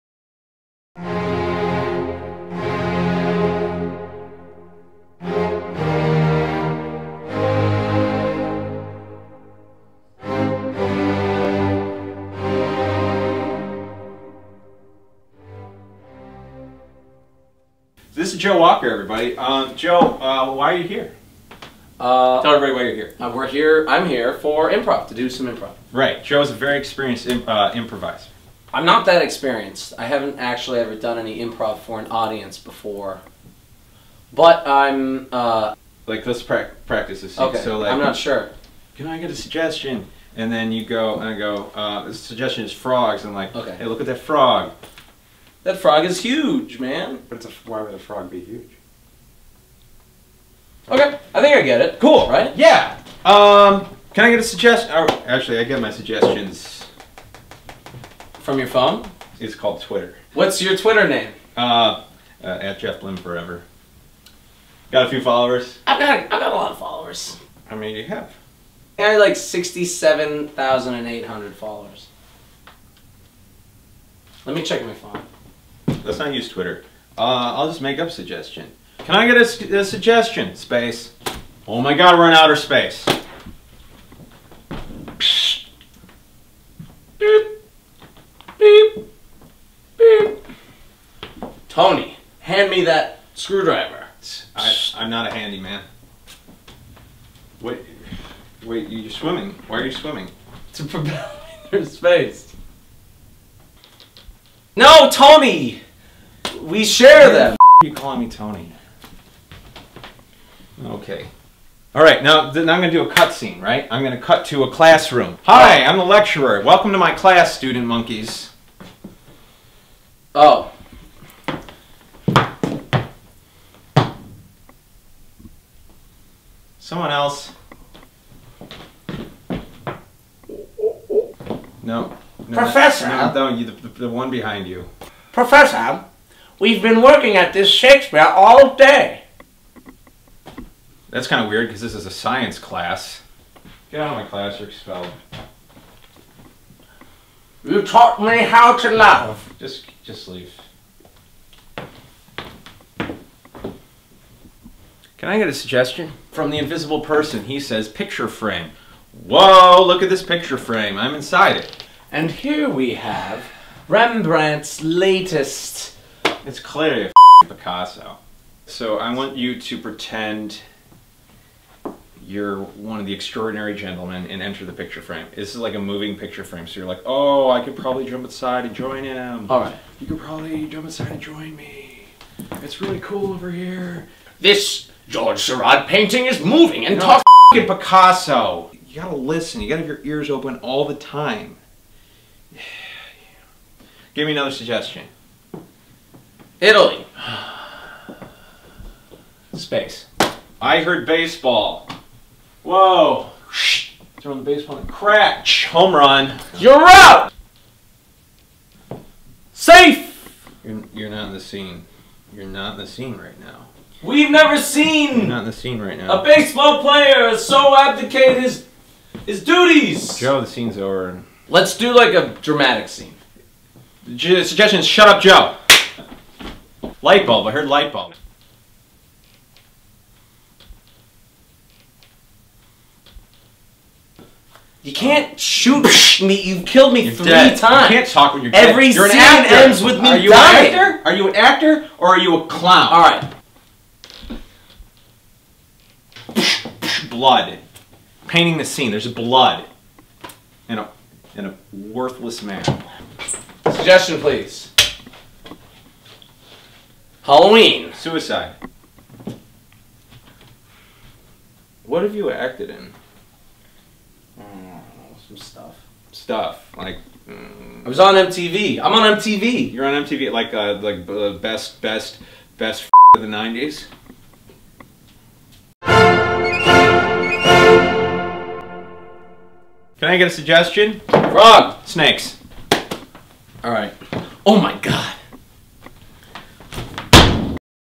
Film. Improv. Now. (Joe Walker)
Joe Walker pays a visit to Jeff's living room, for a little one-on-one showdown.  Heads.  Will.  Roll.

"The funniest YouTube video that's been made by my friends that's not a play that I've ever seen." -- Eric Kahn Gale